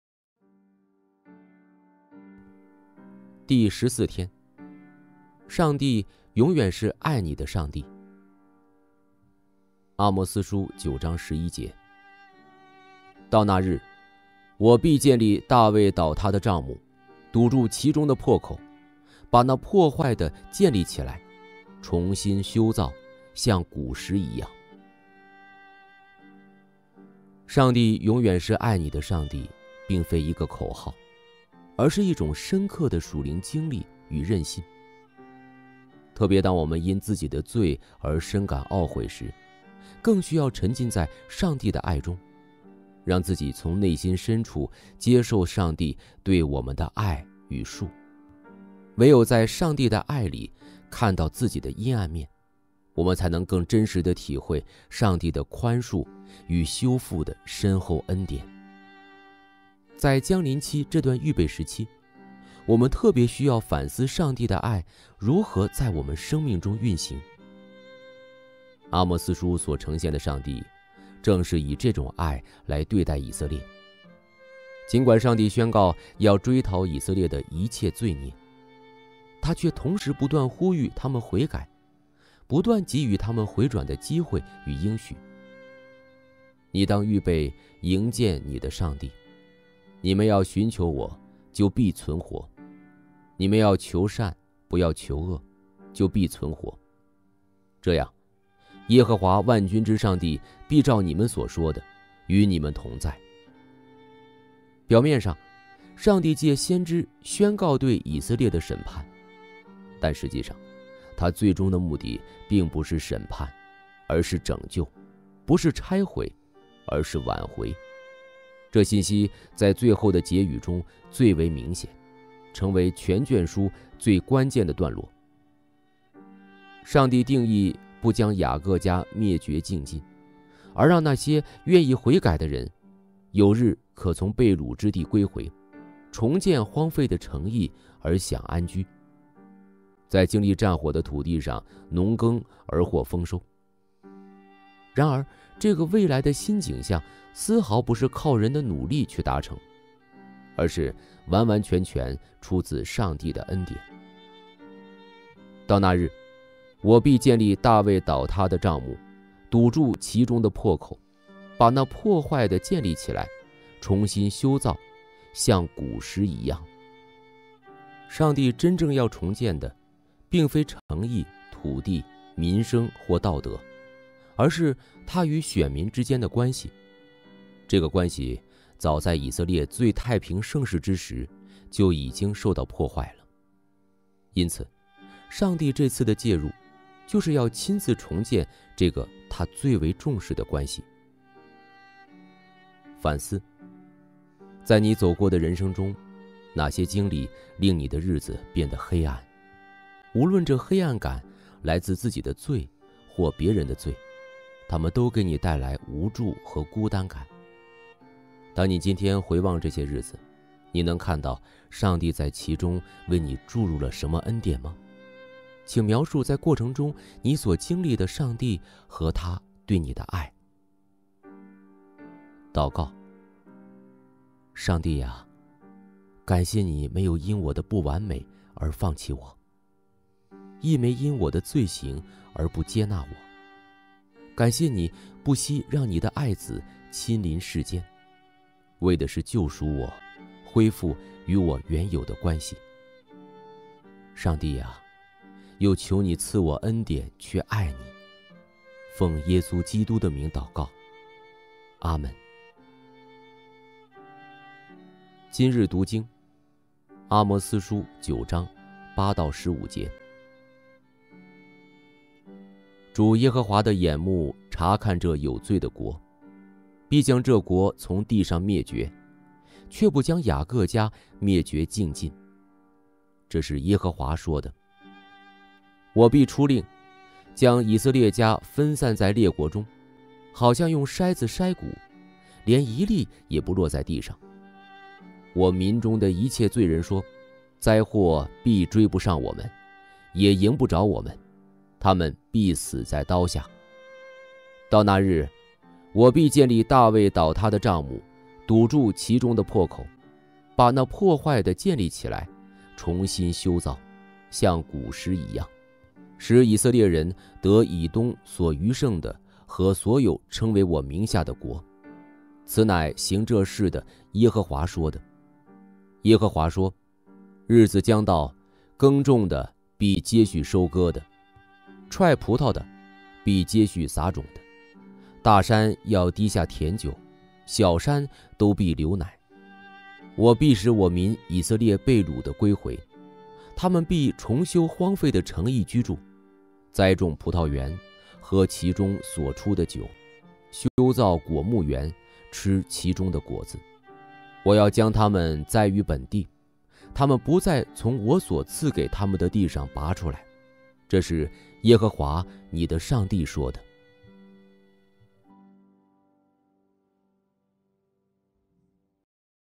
第14天 上帝永遠是愛你的上帝
阿摩司書9章11節：「到那日，我必建立大衛倒塌的帳幕，堵住其中的破口。把那破壞的建立起來，重新修造，像古時一樣。」

在這個將臨期中，當我們再思上帝的寬恕時，願我們更深體會寬恕背後所蘊藏的這份關係。

反思：在你走過的人生中，哪些經歷令你的日子變得黑暗？無論這黑暗感來自自己的罪或別人的罪，它們都給你帶來無助和孤單感。當你今天回望這些日子，你能看到上帝在其中為你注入了什麼恩典嗎？請描述在過程中你所經歷的上帝和祂對你的愛。